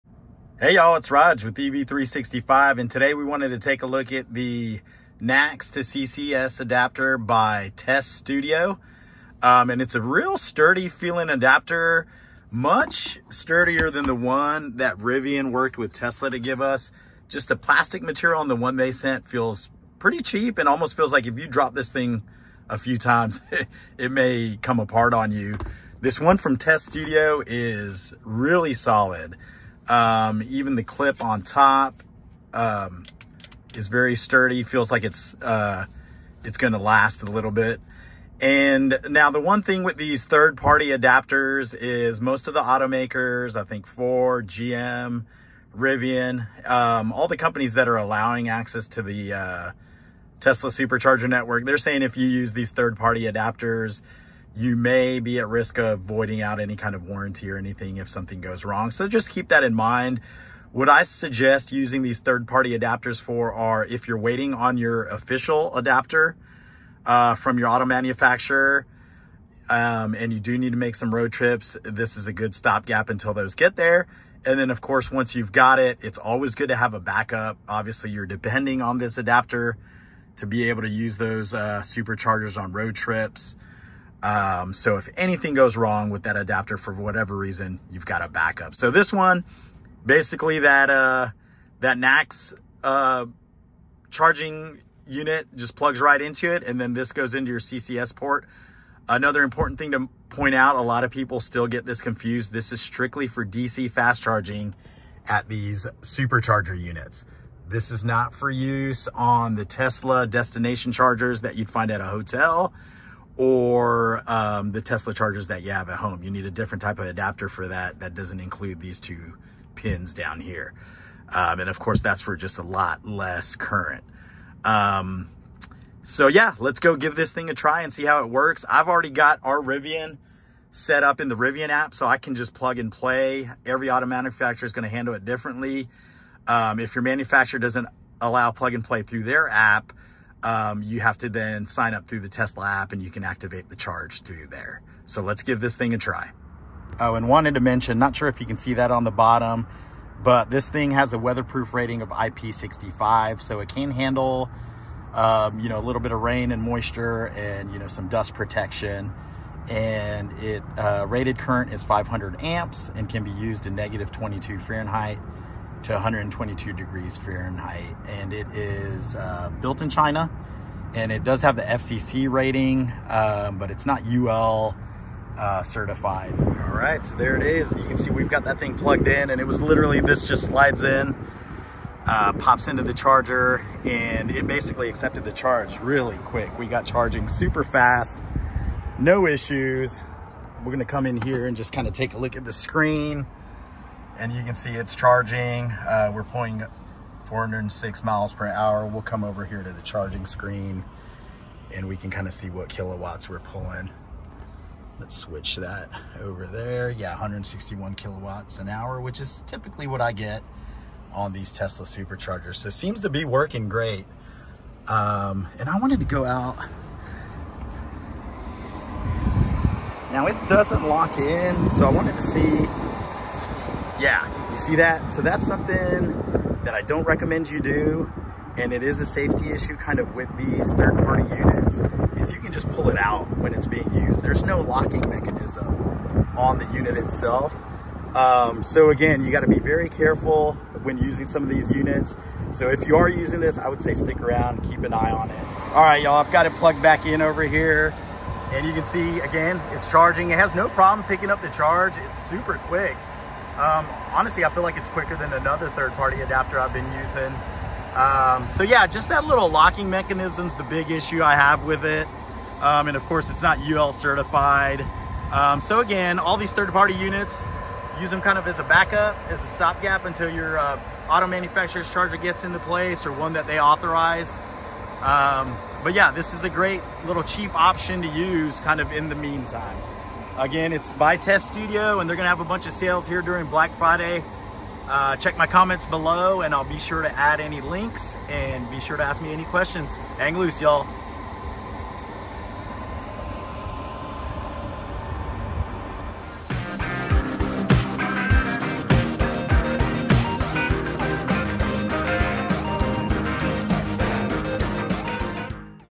TESSTUDIO NACS to CCS Adapter for Supercharger DC Fast Charging - Review
Today we review the TESSTUDIO NACS to CCS Adapter for Supercharger DC Fast Charging sold by TESSTUDIO.  We use it on our Rivian R1T, but this can be used on any non-Tesla vehicle that allows for charging on Tesla's Supercharger Network.

TESSTUDIO sells all types of great accessories for Teslas and electric vehicles.